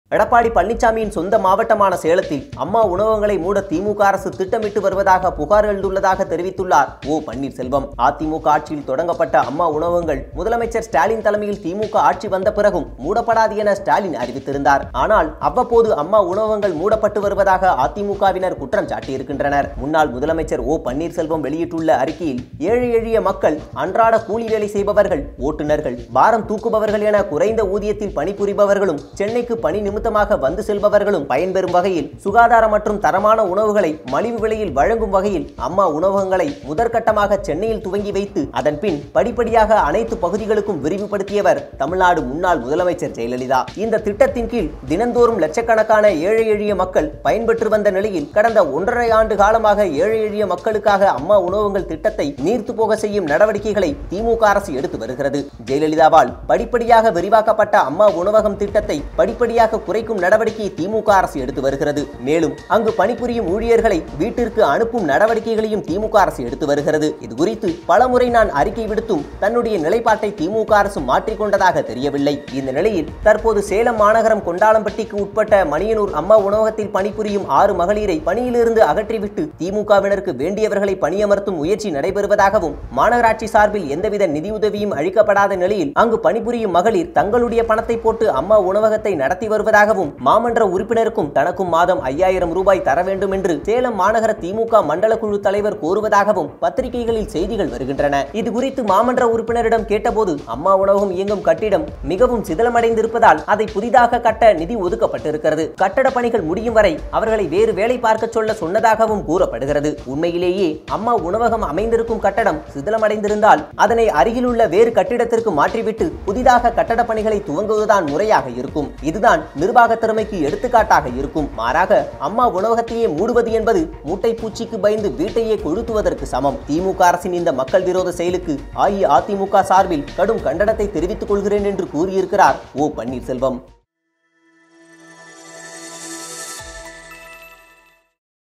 மூட்டைப் பூச்சிக்கு பயந்து வீட்டை கொளுத்துவதற்கு சமம் – OPS
அம்மா உணவகத்தை மூடுவதா OPS கடும் கண்டனம்